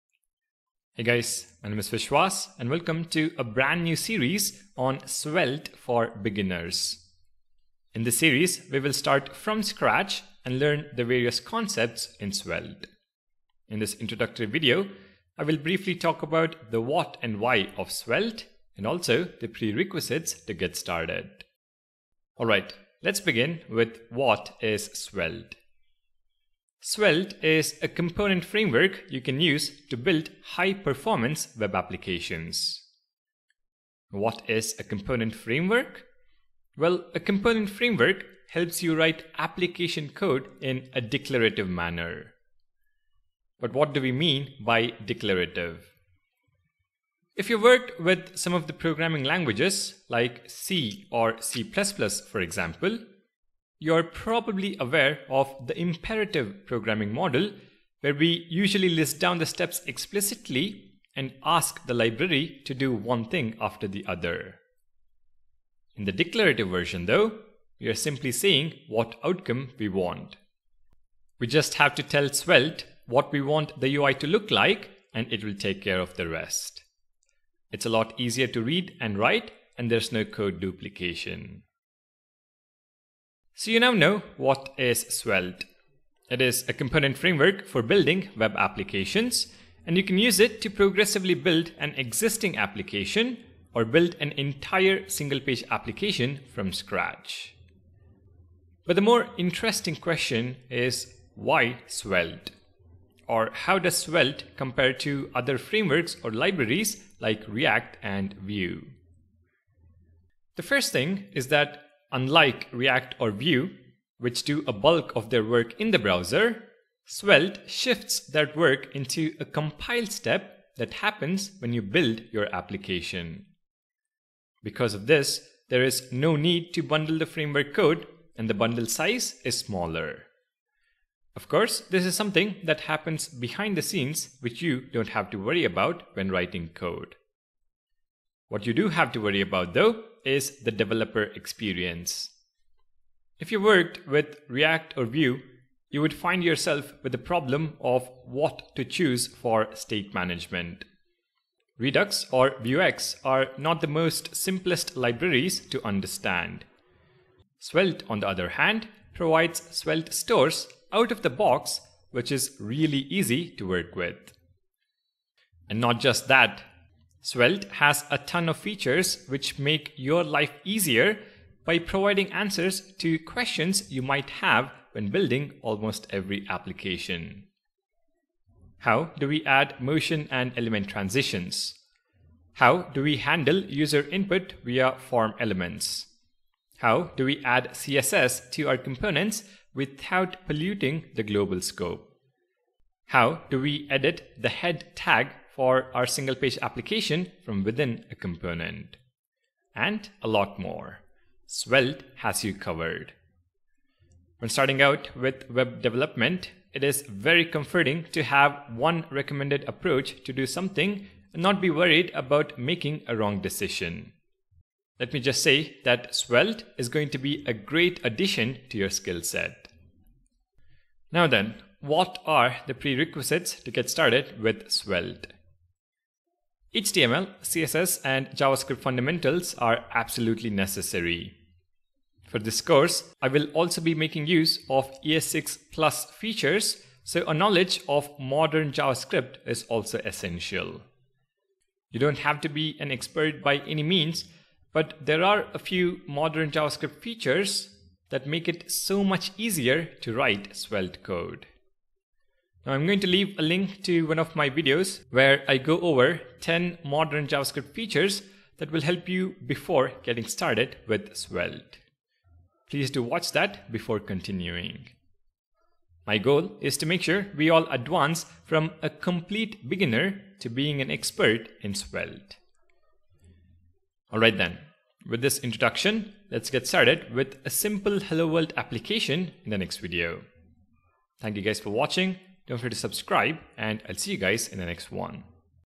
Svelte Tutorial - 1 - Introduction
Svelte Tutorial | Svelte Tutorial for Beginners

Svelte is a tool for building fast web applications.

It is similar to JavaScript frameworks such as React and Vue, which share a goal of making it easy to build slick interactive user interfaces.

But there's a crucial difference: Svelte converts your app into ideal JavaScript at build time, rather than interpreting your application code at run time. This means you don't pay the performance cost of the framework's abstractions, and you don't incur a penalty when your app first loads.

You can build your entire app with Svelte, or you can add it incrementally to an existing codebase. You can also ship components as standalone packages that work anywhere, without the overhead of a dependency on a conventional framework.